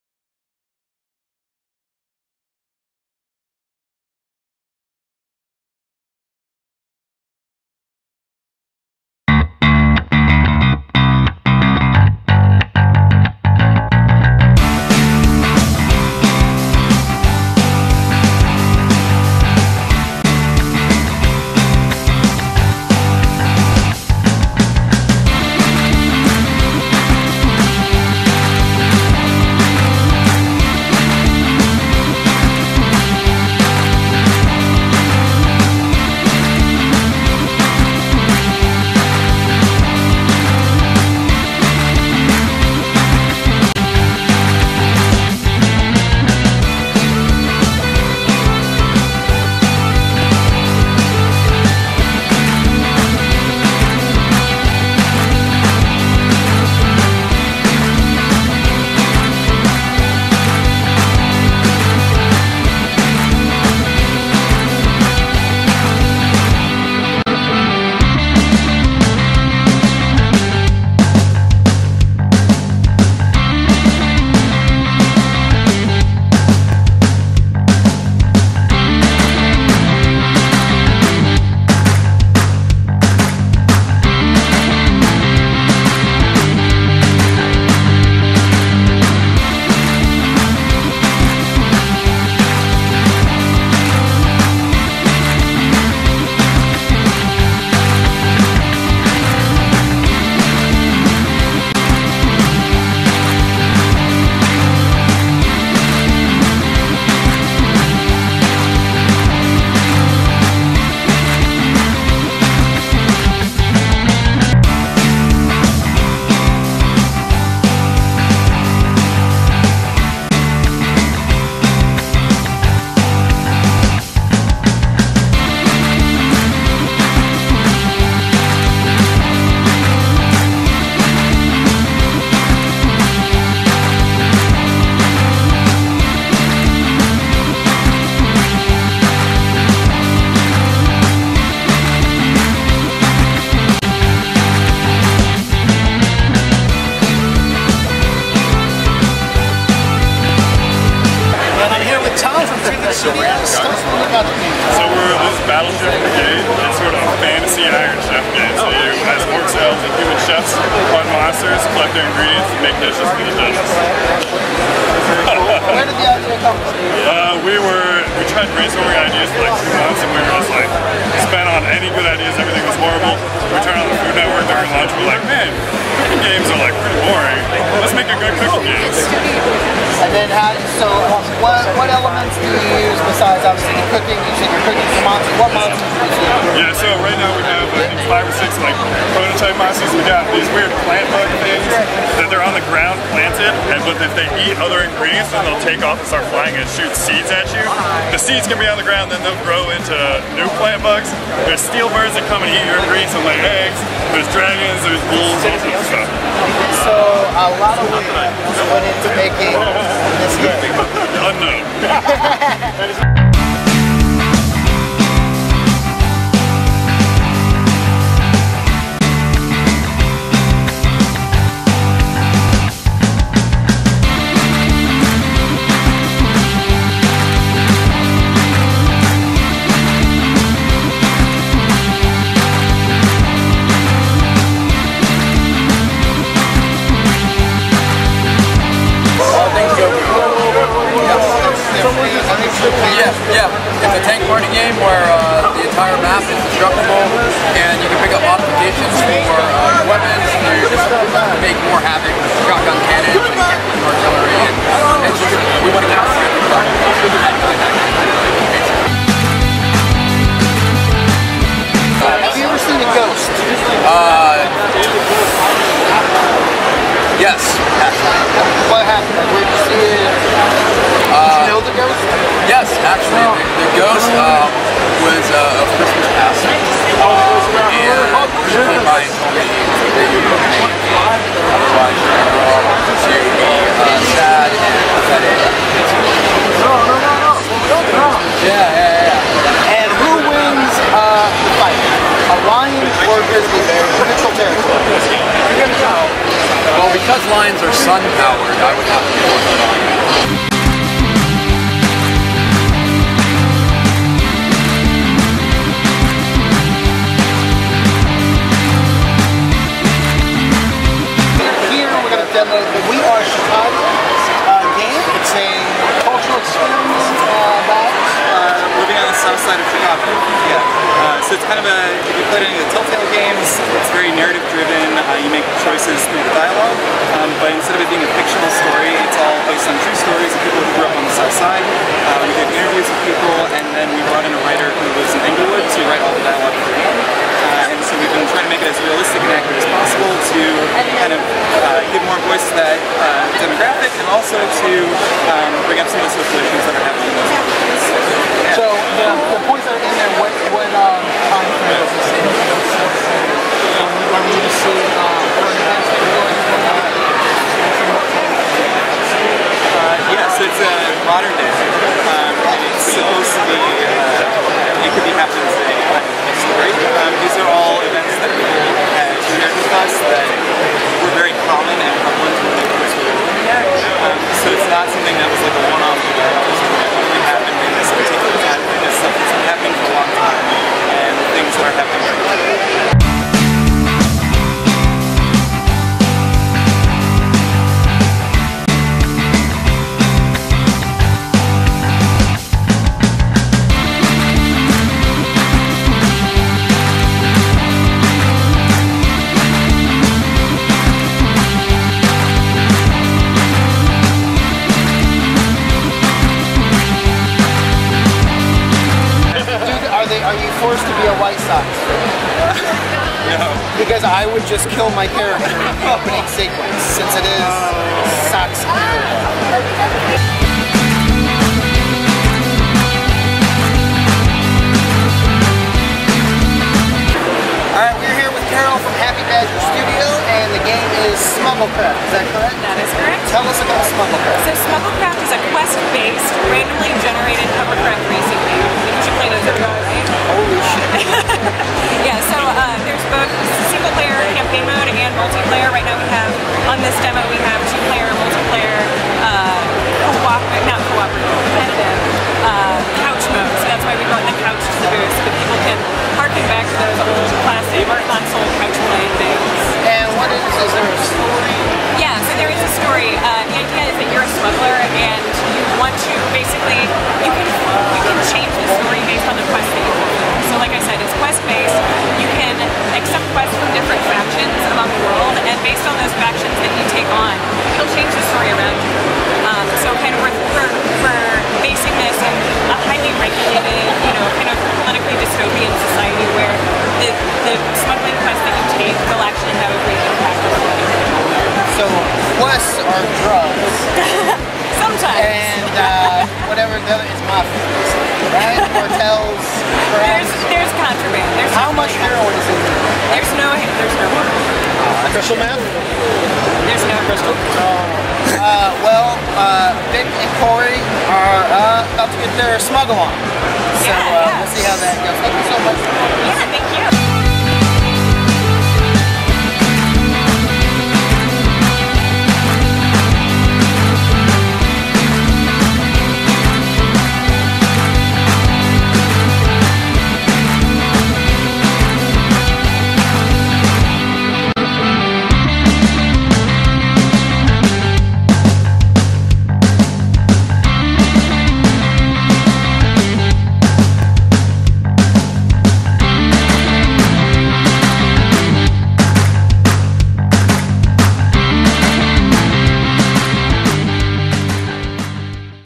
Indy Pop Con 2015
Bi-weekly podcast where Vic takes on Corey in an attempt to convince Brad that their film is better.

We roamed the floor of Pop Con and reviewed the games that were demoed as well.  Some of the audio didn't turn out well, sorry about that but we salvaged as much as we could.